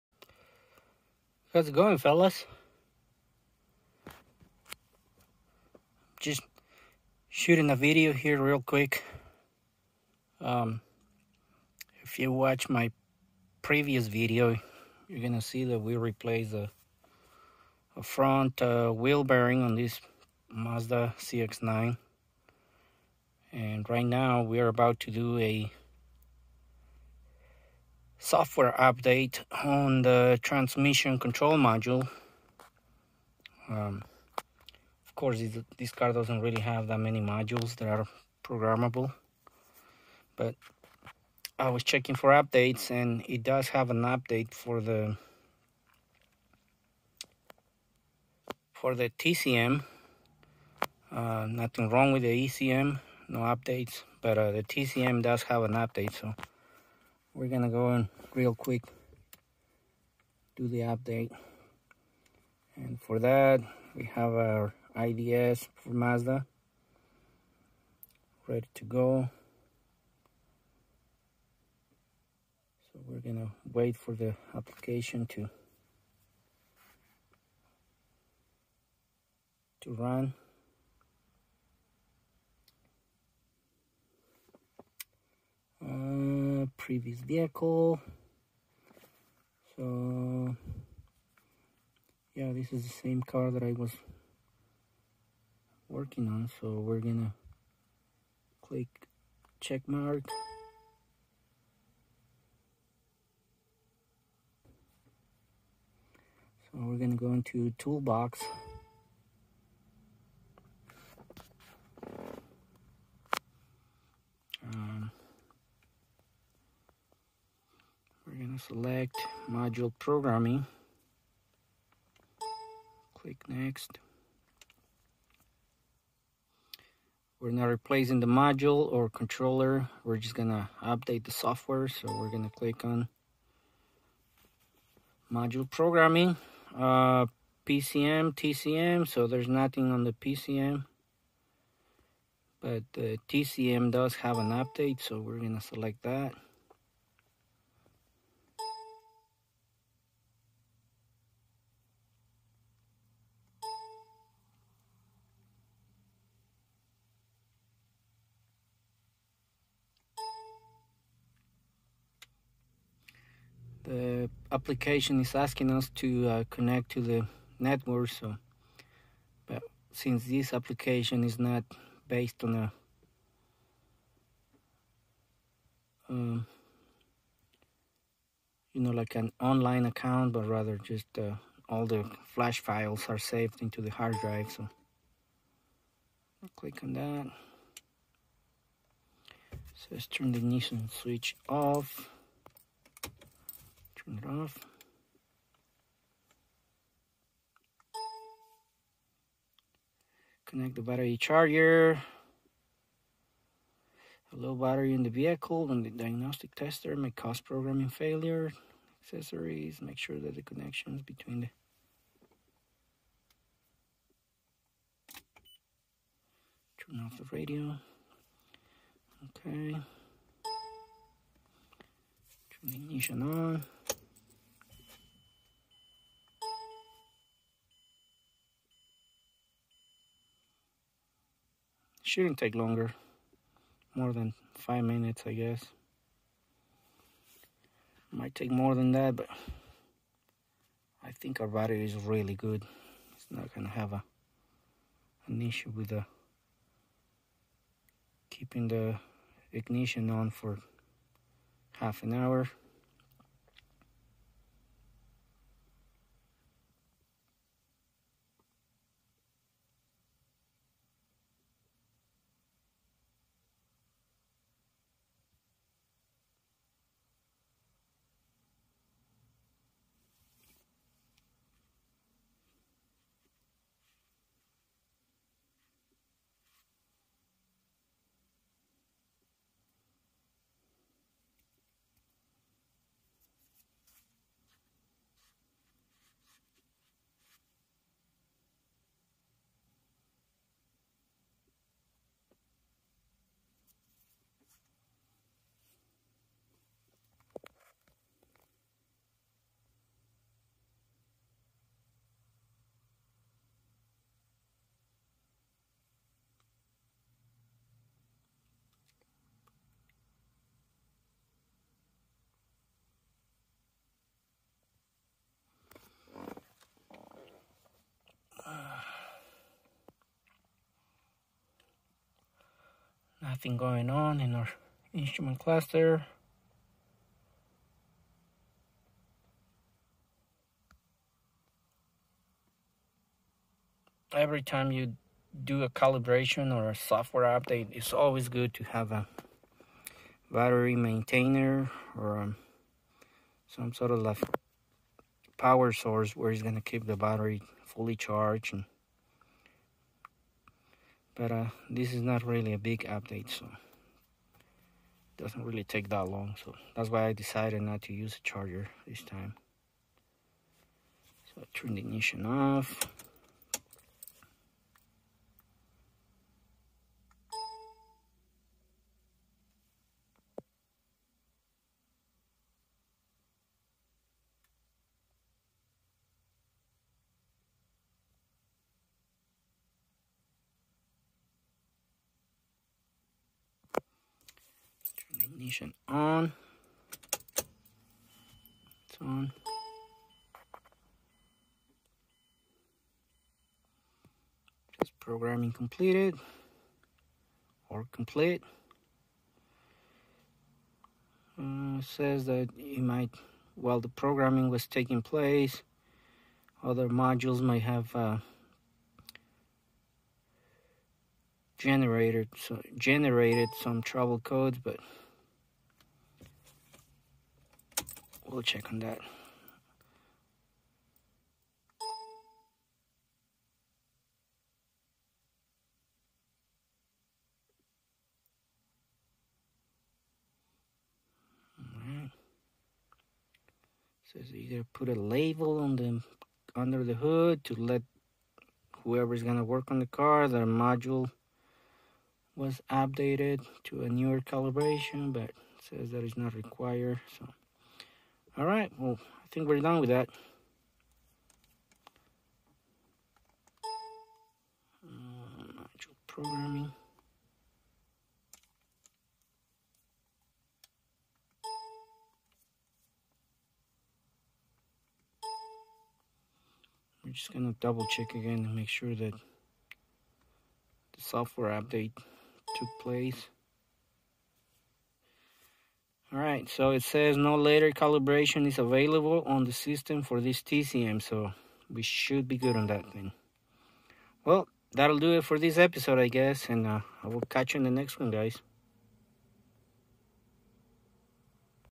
MAZDA CX-9 TCM SOFTWARE CALIBRATION, UPDATE/REPROGRAMMING WITH FACTORY SCAN TOOL MAZDA IDS
JUST DOING A QUICK VIDEO FOR (TCM) TRANSMISSION CONTROL MODULE SOFTWARE UPDATE
OR TCM REPROGRAMMING, THIS IS SAME TRUCK THAT I DID A WHEEL BEARING REPLACEMENT.
CHECK MY OTHER VIDEOS IF YOU WANT TO WATCH IT.